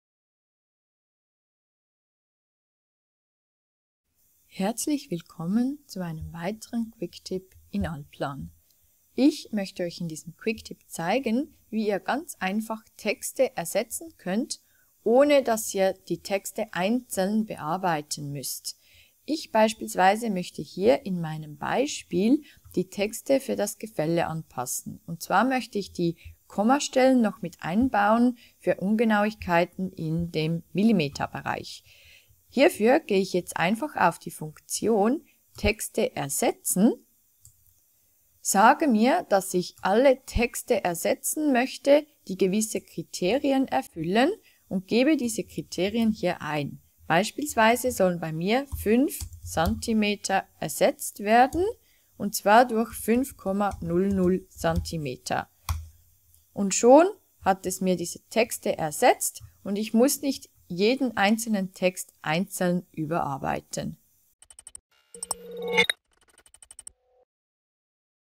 #109 ALLPLAN Quicktips "Texte ersetzten"
In diesem Quicktip zeigen wir wie ganz einfach Texte gesucht und ersetzt werden können.